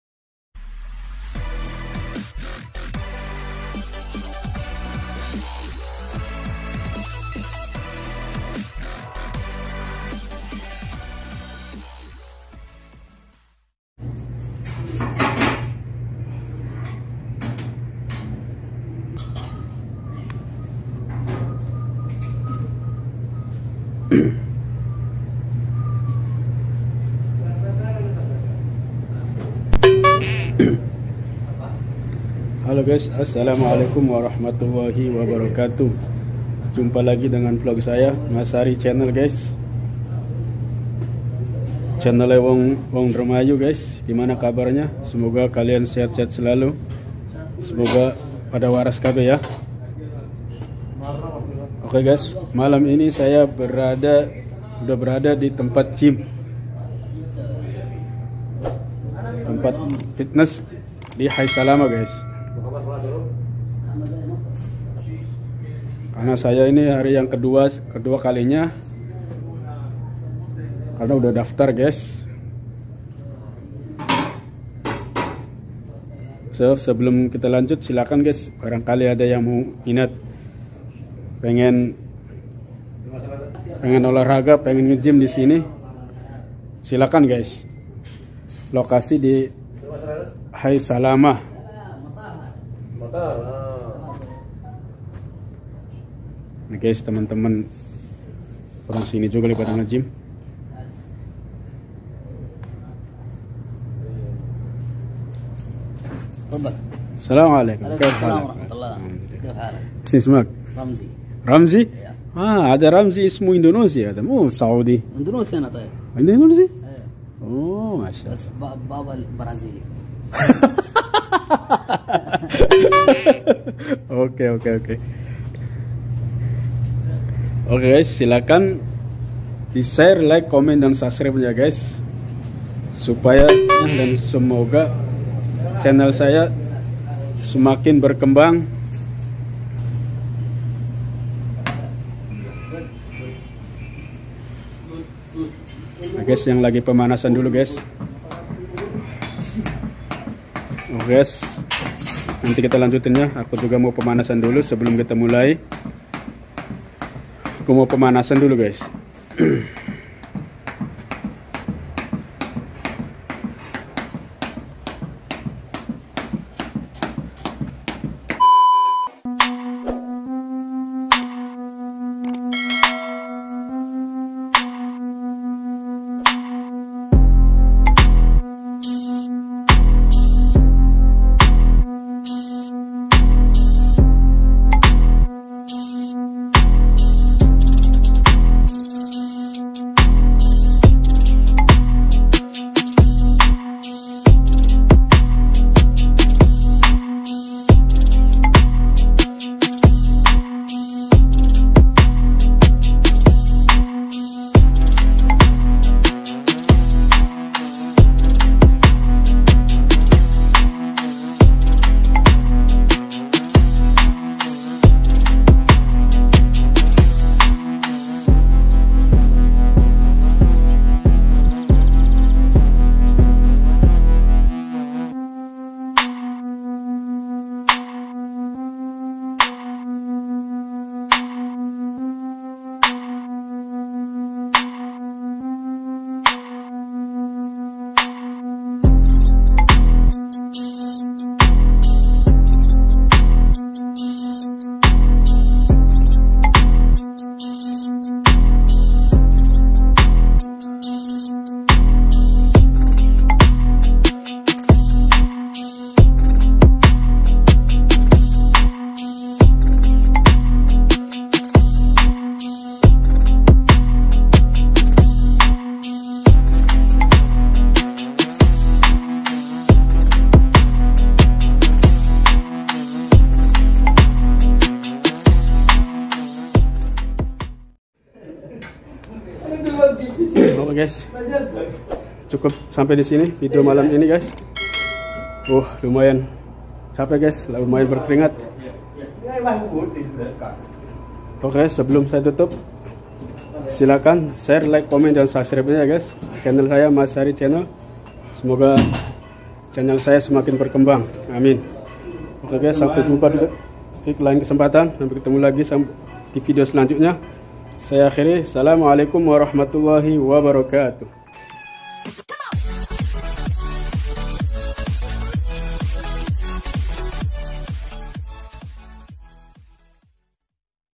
GYM FITNESS PEMANASAN MEMBAKAR BEBERAPA KALORI
Video ini saya lagi gym fitness pemanasan membakar beberapa kalori